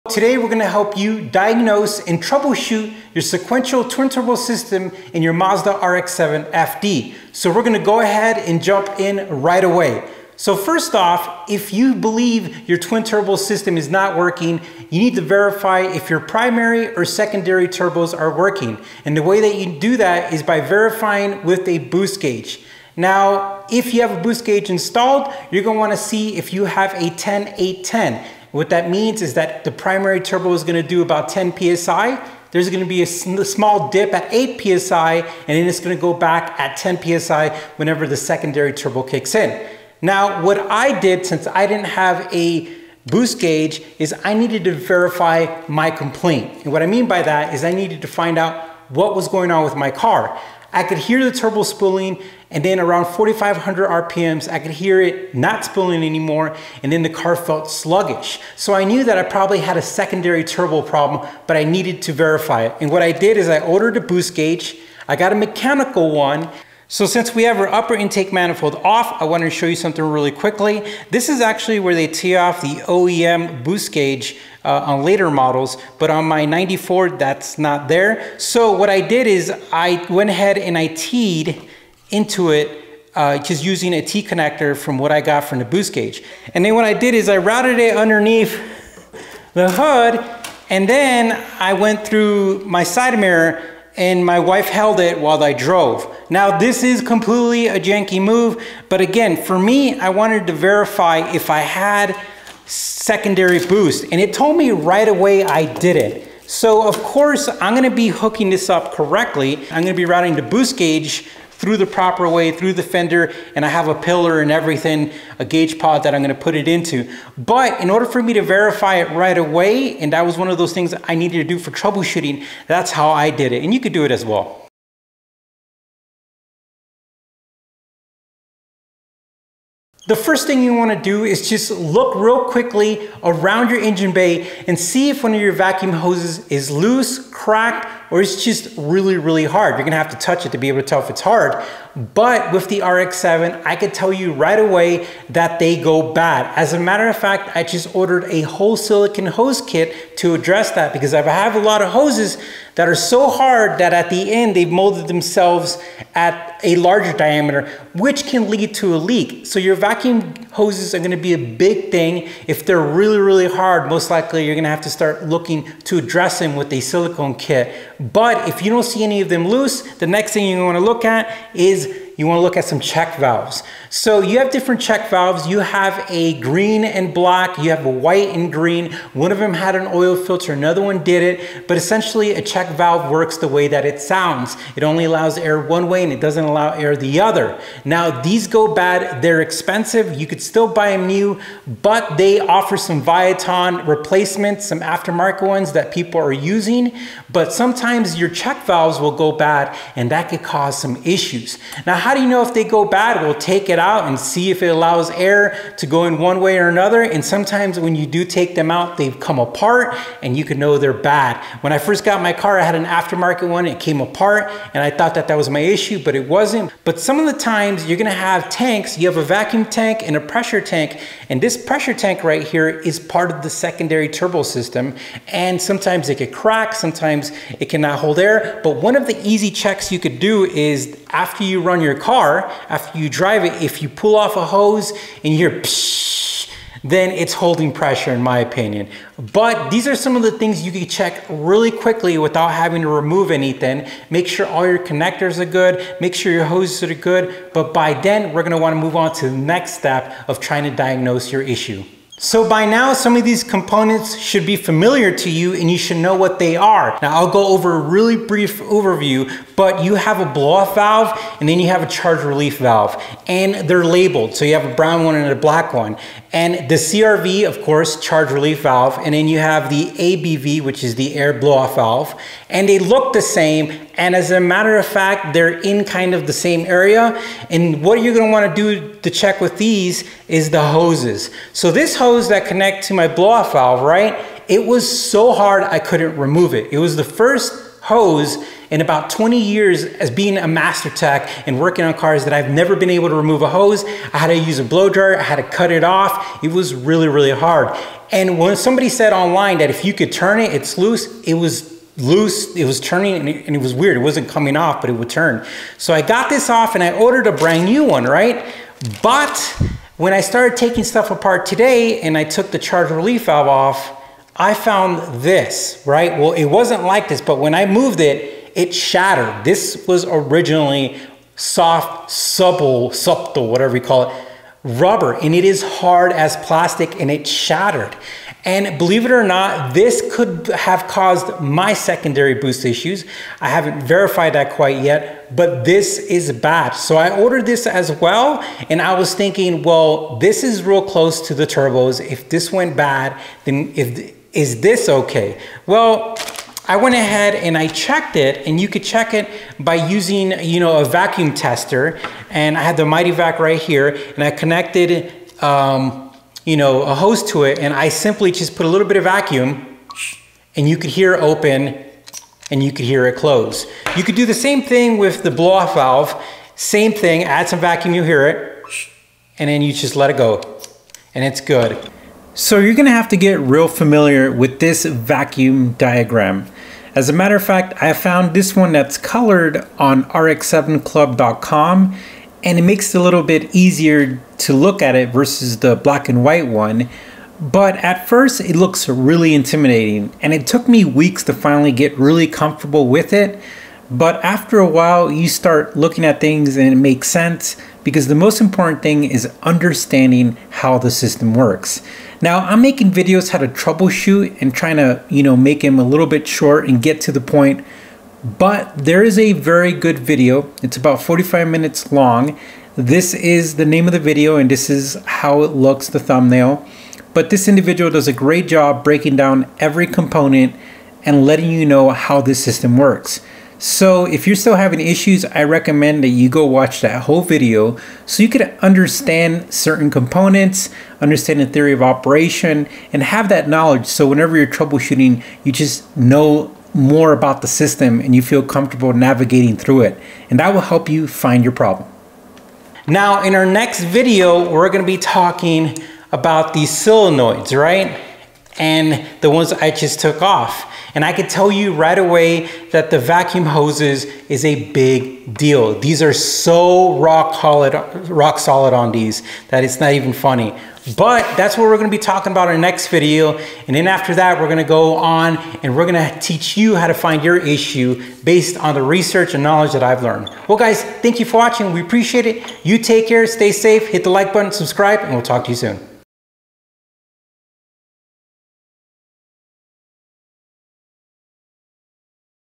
FD Mazda RX-7 Sequential Turbos Troubleshooting - Pt.1(Boost Check, Hoses, Check Valves)
In this video we help diagnosis and troubleshoot the sequential twin turbo system in the Mazda RX-7 FD model with the 13B rotary engine. This was filmed during a live issue with my RX-7. The second turbo was not kicking on. I had primary turbo boost but after the changeover I got nothing. I will video the process till I find a solution or come to a conclusion.